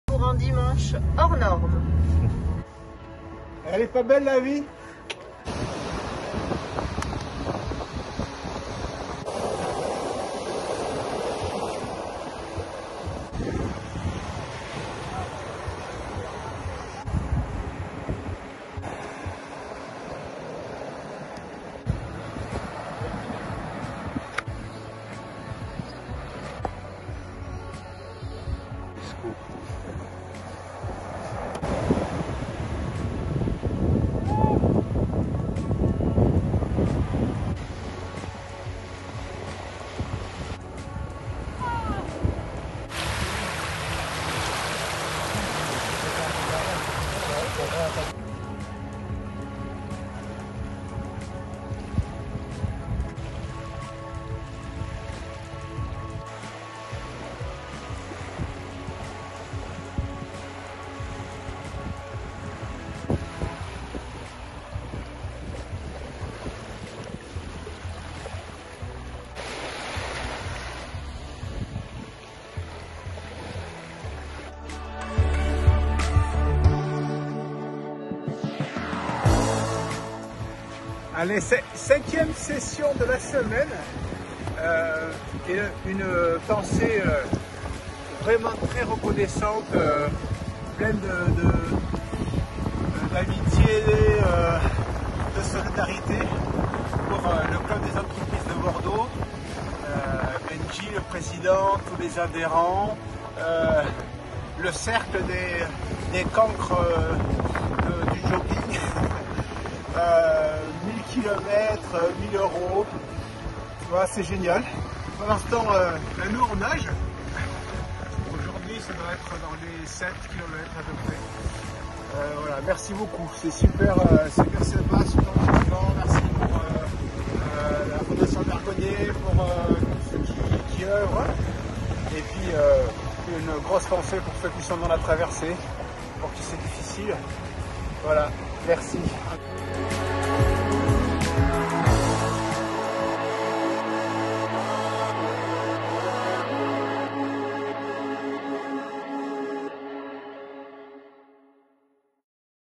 Session d'entrainement Traverser-Vivre 21 mars 2021
Retour en images de la session 🏊‍♂️ de ce Dimanche printanier 🌞 mais frisquet 😉
Petite dédicace vidéo au Club des Entreprises de Bordeaux, ses adhérents et son Président Benjamin Reverdy pour l'opération des 1000 kms 🏃‍♂️🏃‍♀️🏃 1000 €uros !